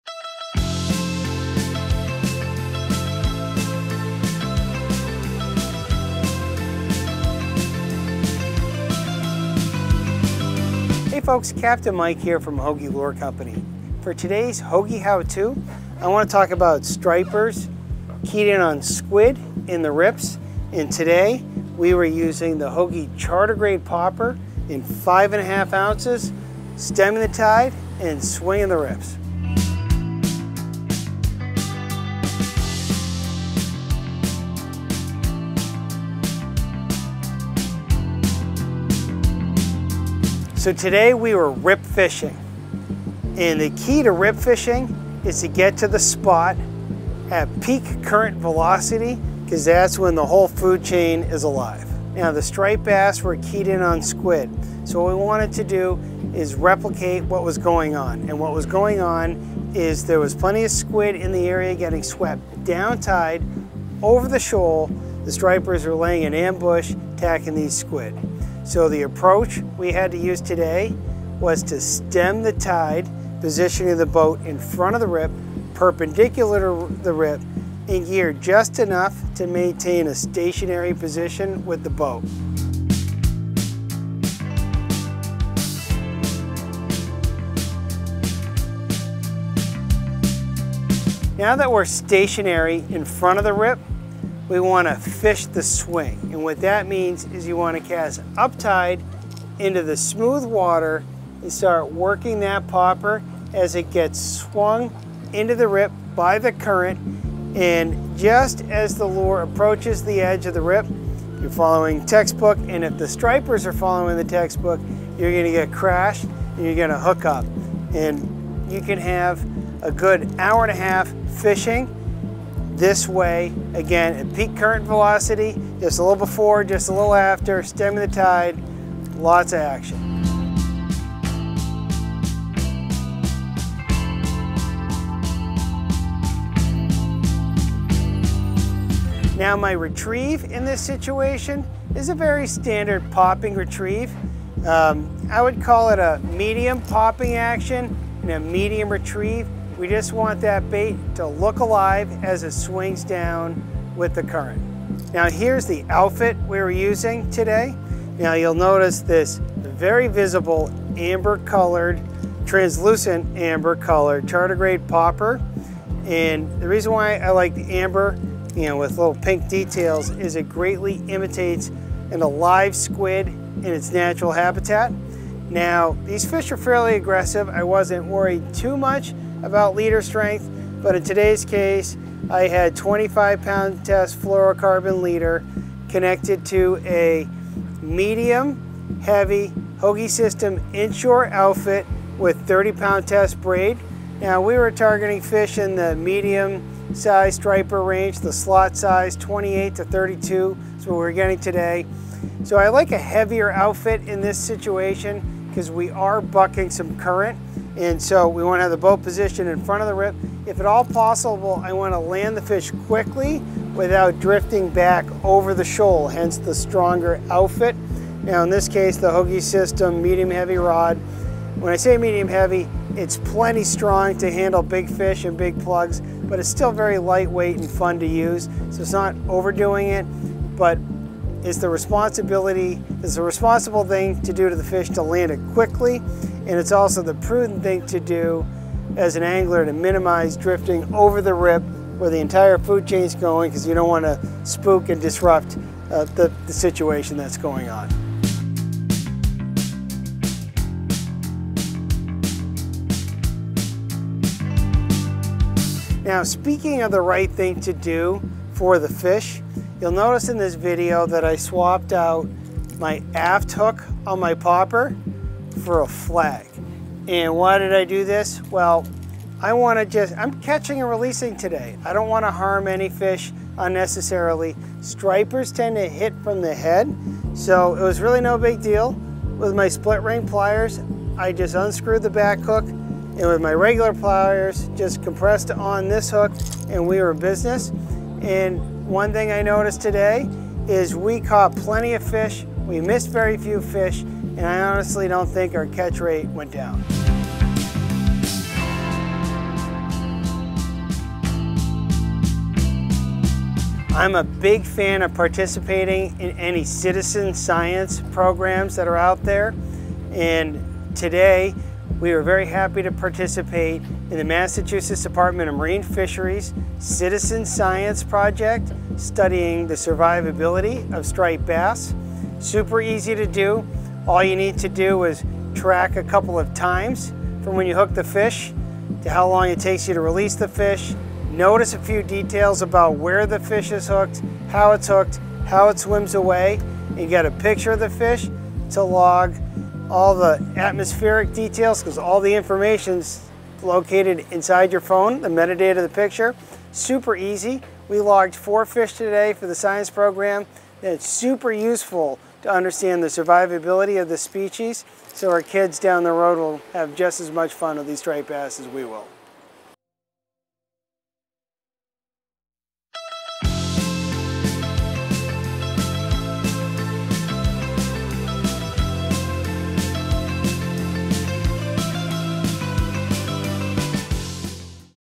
Hogy How-To: Stripers Popping Squid
🎣 Introduction: Captain Mike from Hogy Lure Company discusses techniques for catching striped bass using the Hogy Charter Grade Popper.
🌊 Rip Fishing Strategy: The key to rip fishing is to be at the spot during peak current velocity when the food chain is most active.
🦑 Squid Replication: Stripers target squid, so replicating squid with amber-colored poppers yields better results.
📍 Boat Positioning: Positioning the boat perpendicular to the rip allows for effective casting into the smooth water and the ability to stem the tide.
🎣 Fishing Techniques: Use a medium popping retrieve and a medium heavy rod with at least 20 lb fluorocarbon leader and 30 lb braid for targeting medium-sized stripers.
🐟 Catch and Release: Replacing the back hook with a flag reduces harm to the fish during catch and release. It also reduces the time spent de-hooking the fish, so you can spend more time casting!
📊 Citizen Science: Participation in the Massachusetts Department of Marine Fisheries' citizen science project helps track the survivability of striped bass.

Insights Based on Numbers
Peak Current Velocity: Fishing is best during peak current velocity, increasing the likelihood of catching stripers.
Slot Size: Targeting 28-31 inch stripers requires strong yet lightweight gear to handle the current and minimize drift.

Chapters
Introduction - 00:00
Captain Mike introduces the topic of stripers keening on squid in the rips and using Hogy Charter Grade Poppers.

Rip Fishing Strategy - 00:38
Explanation of the importance of fishing during peak current velocity.

Squid Replication Technique - 01:08
Discussion on how to replicate squid using amber-colored poppers.

Boat Positioning - 01:48
Tips on how to position the boat perpendicular to the rip for effective fishing.

Fishing Techniques and Gear - 02:28
Details on the medium popping retrieve, rod setup, and line specifications for catching medium-sized stripers.

Catch and Release Best Practices - 03:38
How replacing the aft hook with a flag helps in reducing harm to fish during catch and release.

Citizen Science Participation - 05:03
Encouragement to participate in the Massachusetts Department of Marine Fisheries' citizen science project to track striper survivability.